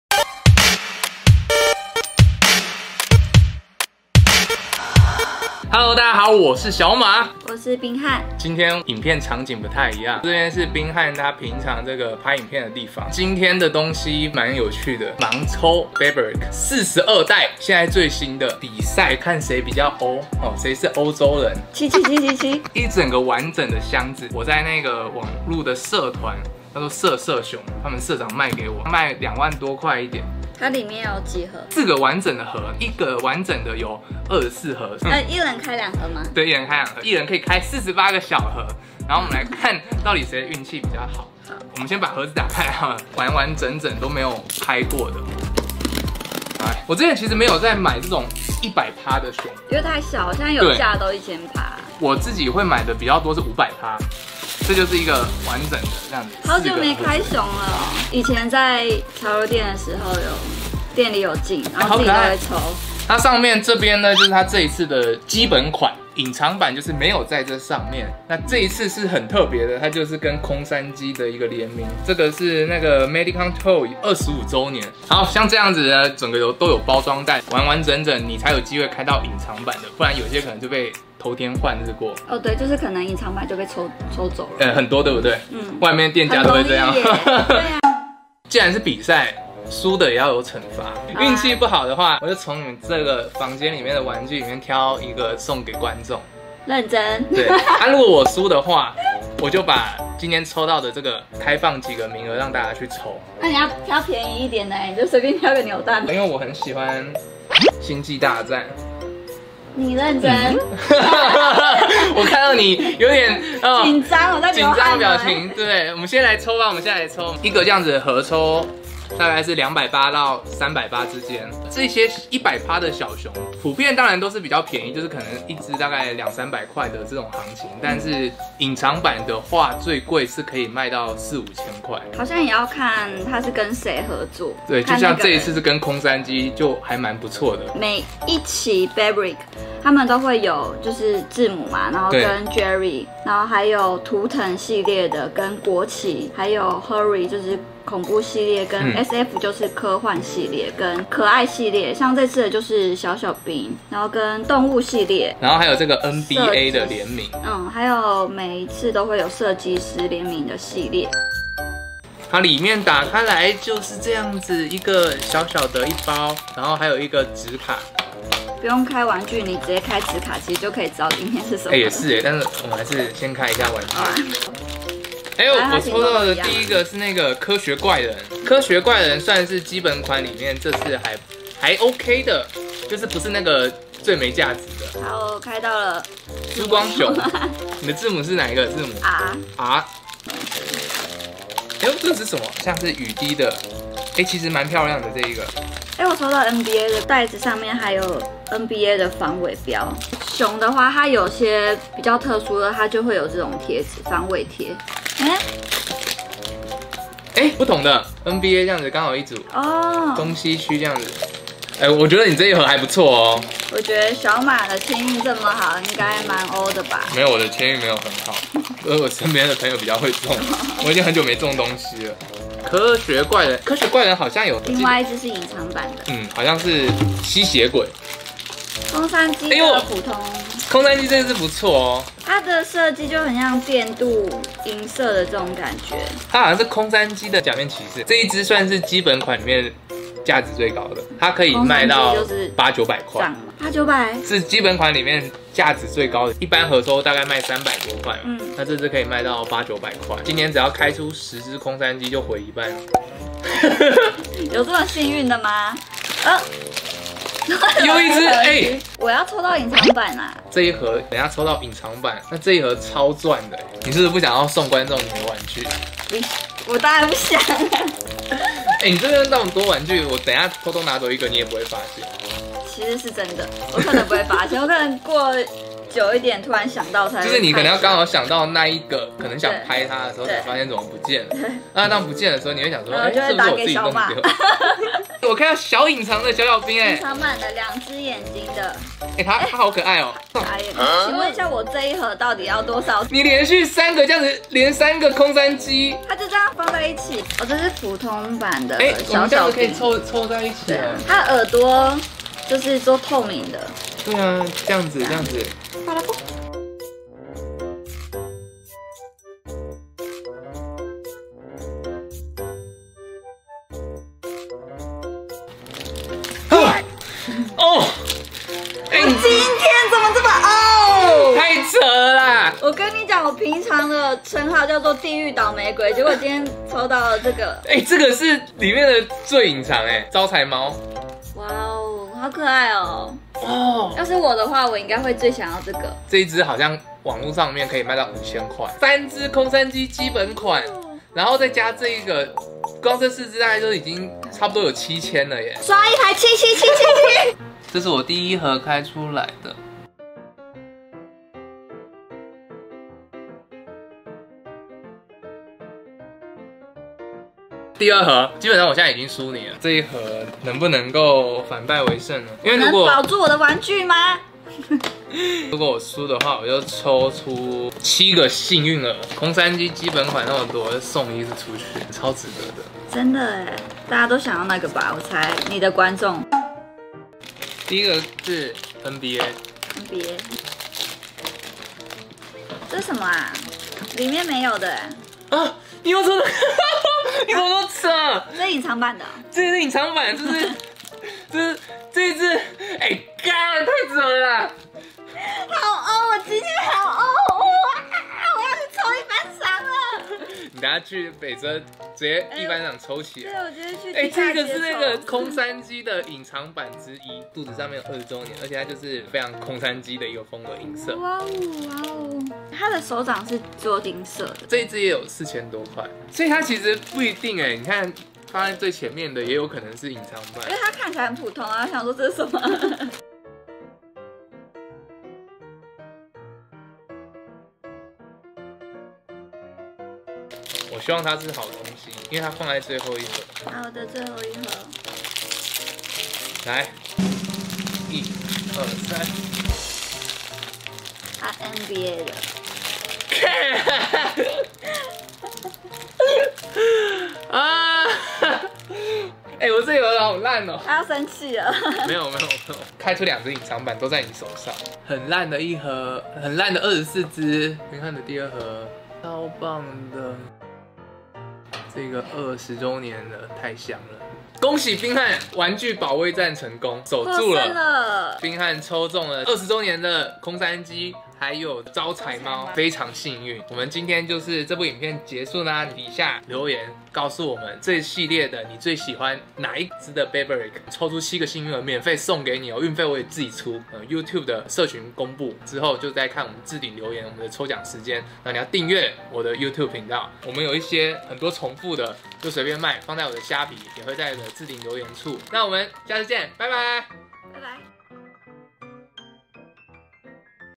最新Be@rBrick 42代盲抽！一隻250塊的小熊竟然能賣到5000塊 ft. @cookie88979 ｜XiaoMa小馬​
今天盲抽be@rbrick 42代
跟玩具界的女神 賓漢一起來比賽
到有誰會贏呢？
若有廠商想贊助我們,或合作影片可以透過下面連結聯絡